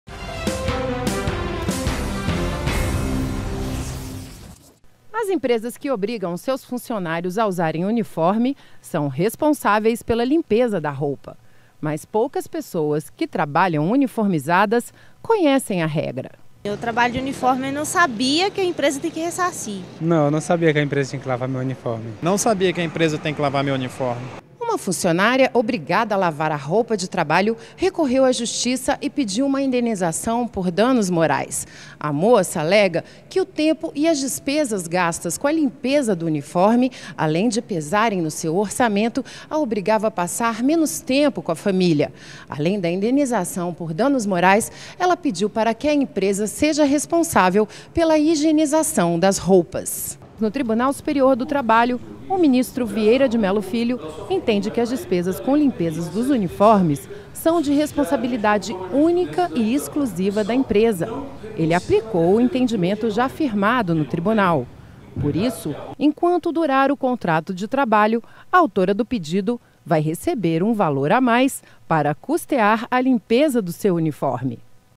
Empregada do supermercado Extra receberá despesas com lavagem do uniforme
Uniforme no trabalho: obrigação ou necessidade? Muitos funcionários  não gostam desta exigência, mas mesmo assim têm que usar e zelar pela roupa.  Já a responsabilidade com as despesas e lavagem do uniforme é da empresa, no entendimento do Tribunal Superior do Trabalho.